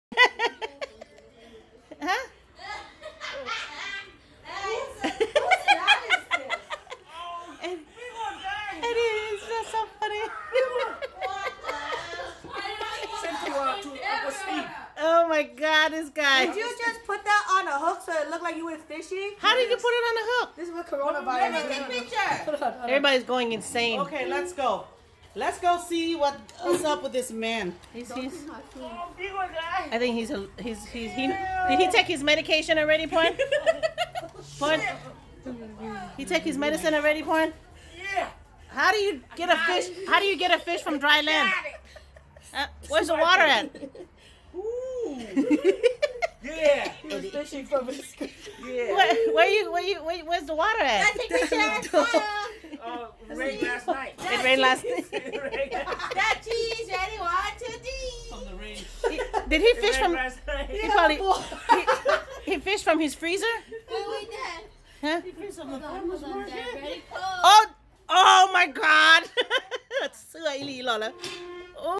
Eddie gone insane. Quarantine sucks!
This is what happened when we stay home to damn long.